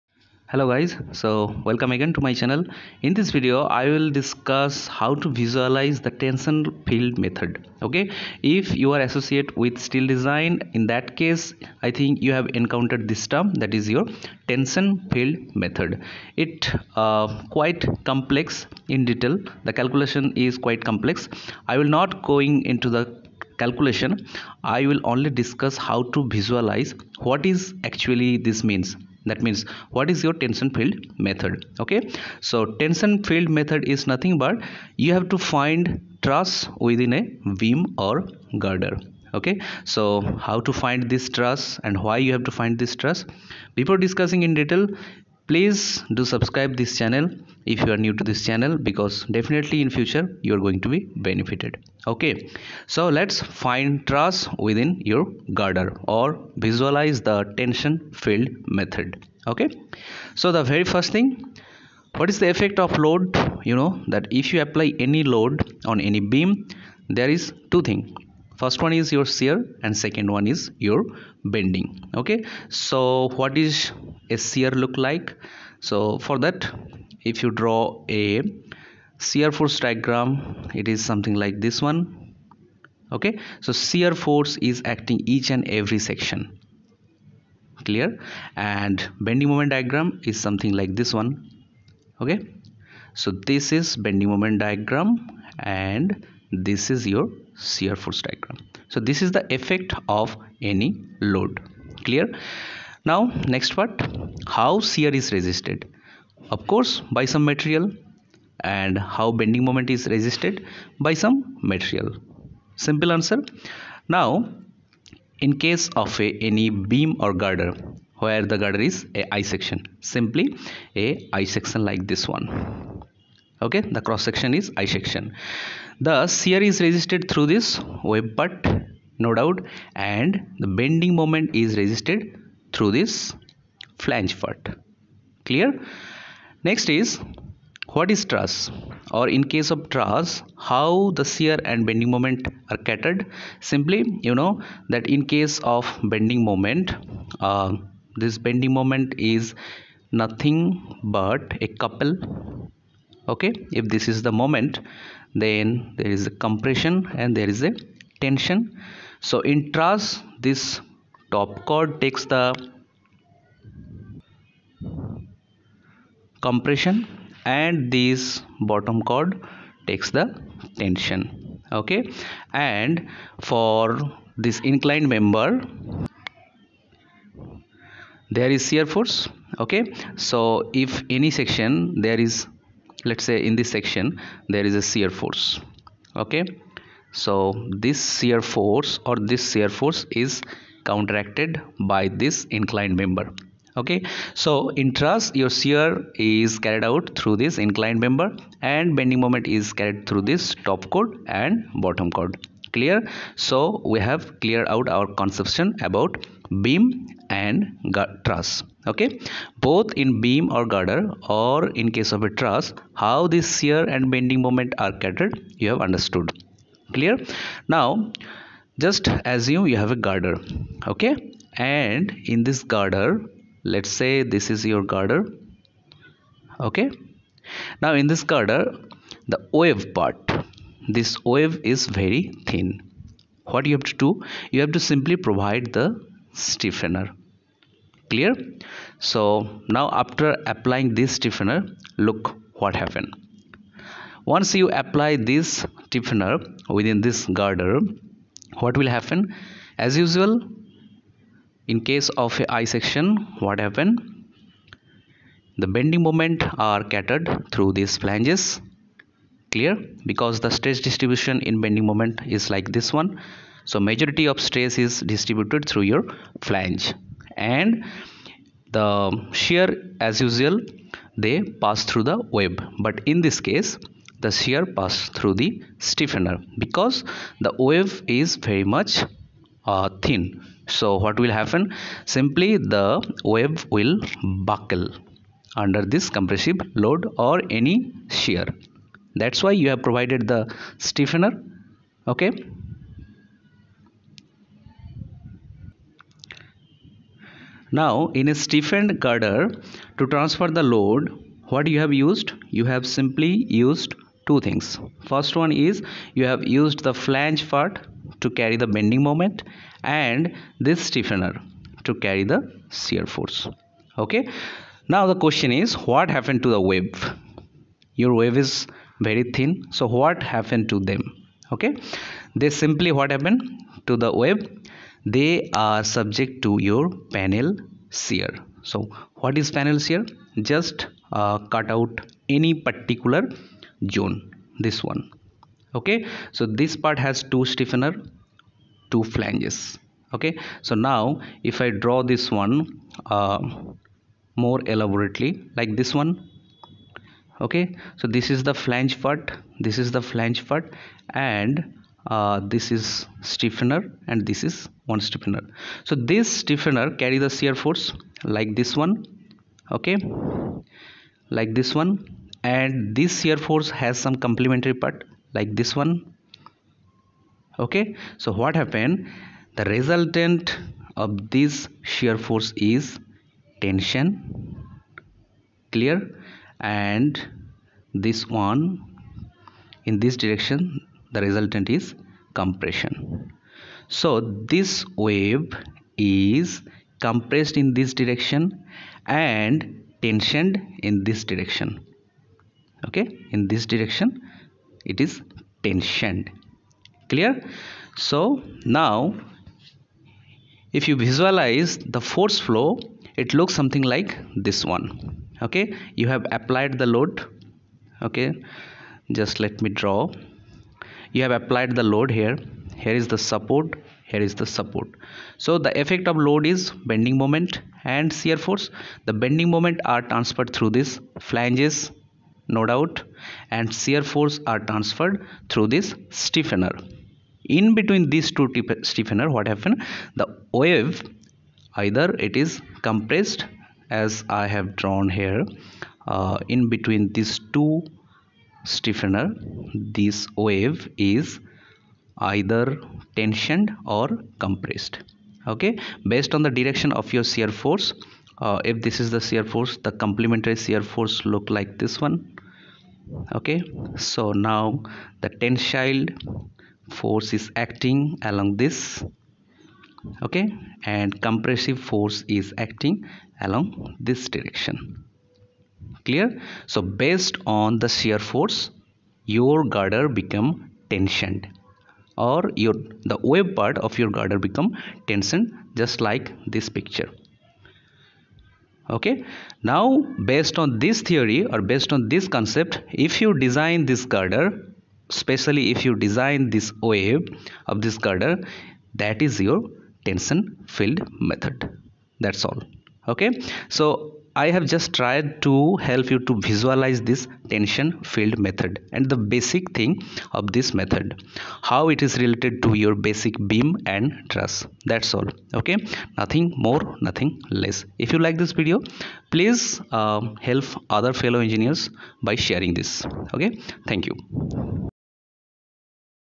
Visualization of Tension Field Method
This video is to provide you a concept of Tension Field Method. After watching this video you will be able to visualise the Tension Field Method.